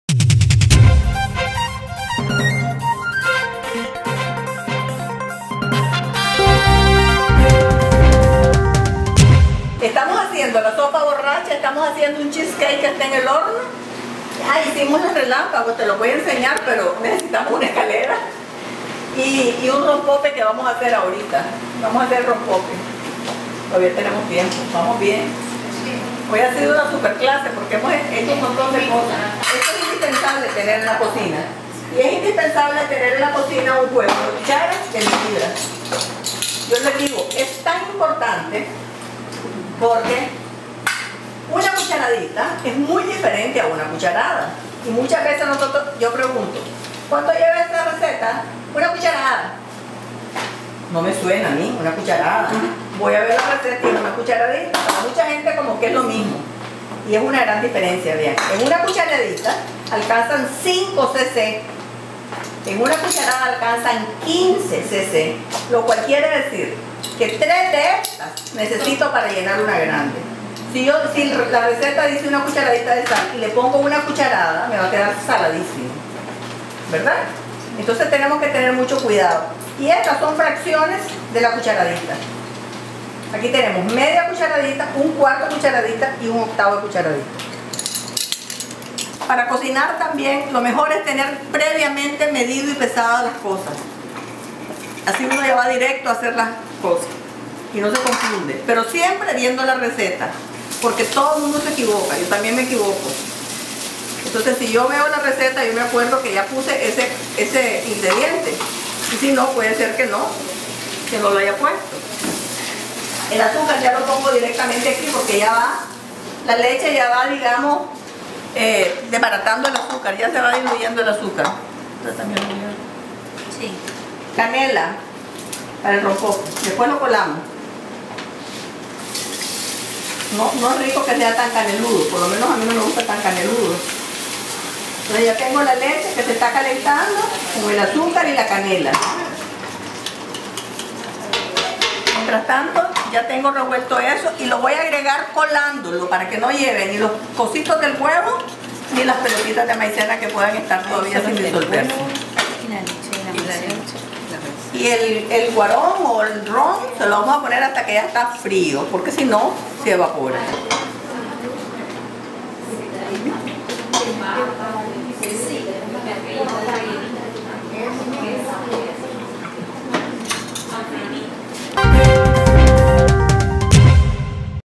Vida y pasión en la cocina
Josefina Gurdián Mántica, o Pinita, como se le conoce, era una chavala cuando se metió a la enseñanza de la cocina. Vivía en su natal León. Hoy aquí, varias décadas después, continúa con el mismo entusiasmo. "Siempre me ha gustado enseñar, transmitir lo que sé", cuenta. BISMARCK PICADO / END